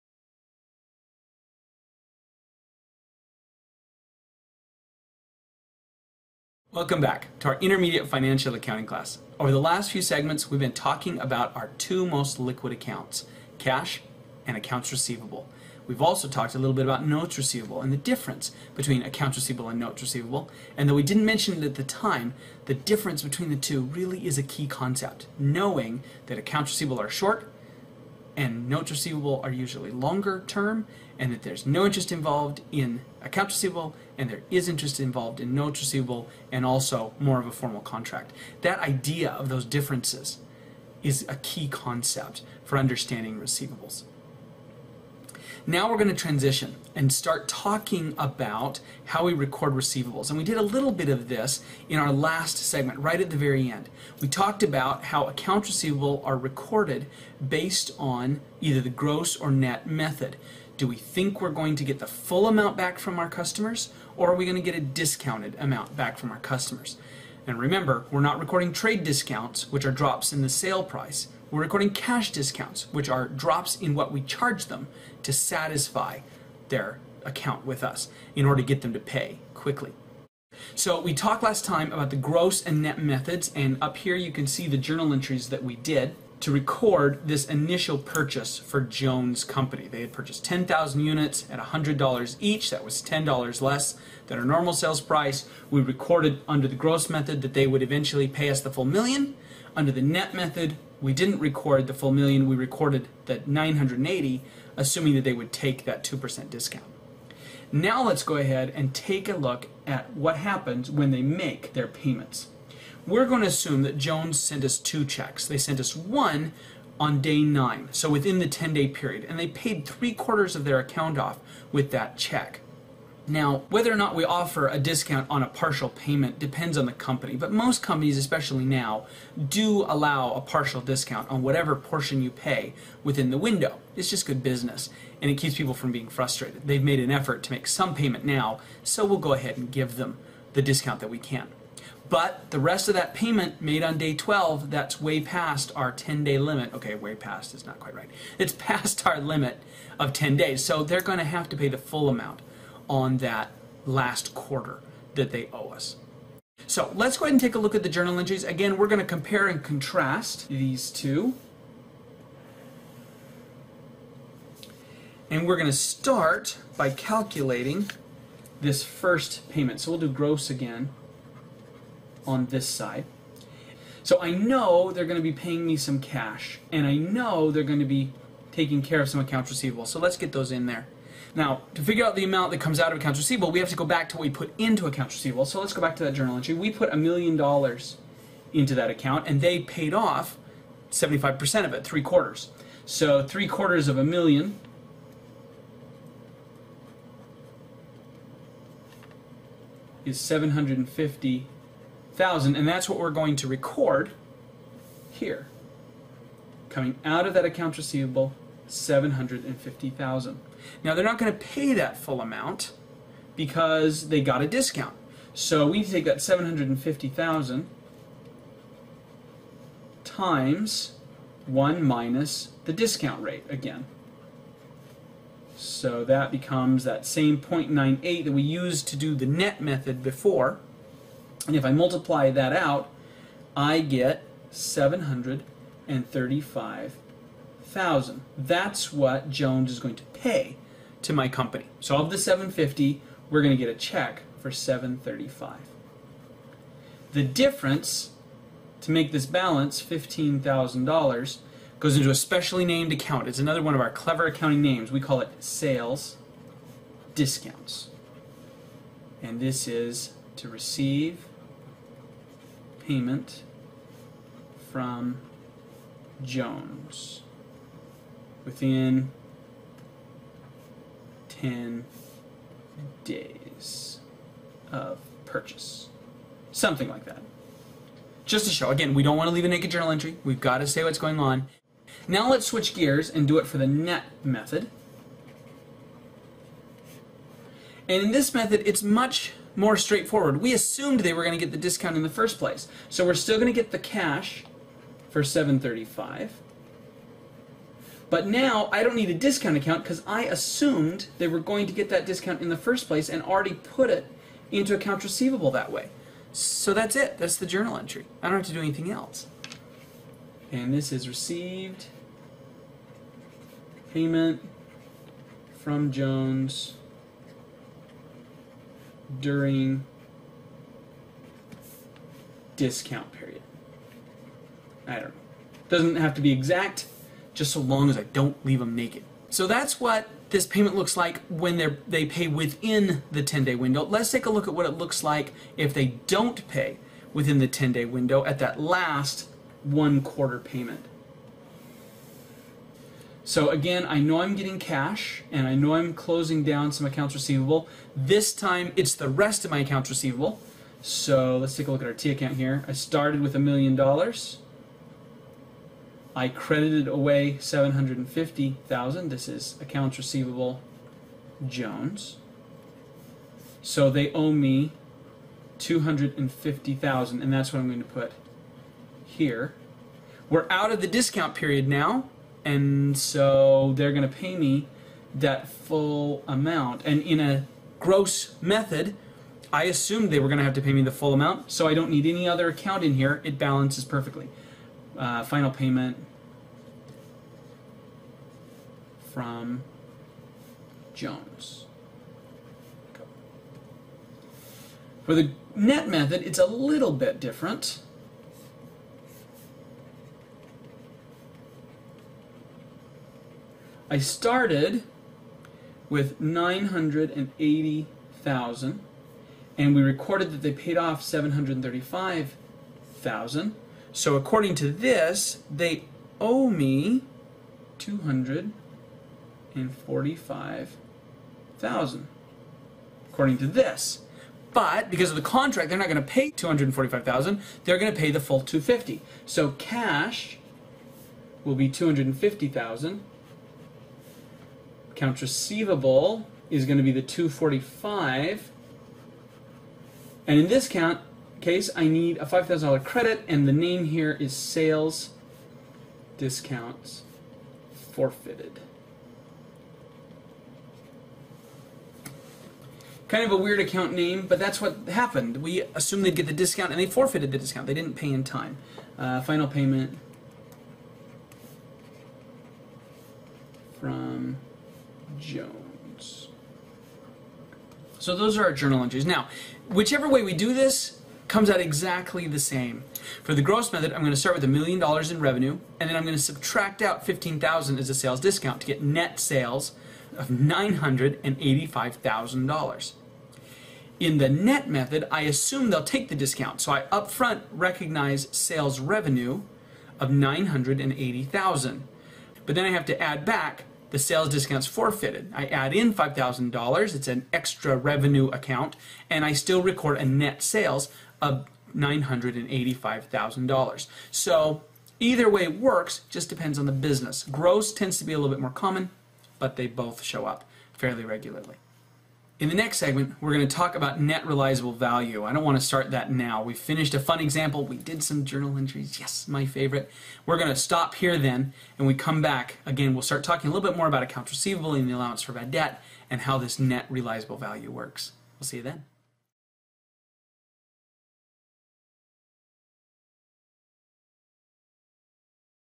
ABC Recording Cash and Accounts Receivable pt 3
This 3rd segment continues our discussion of the gross and net method for recording accounts receivable. We add on to the example we started in segment 2, showing the journal entries for receiving payments on account under both methods.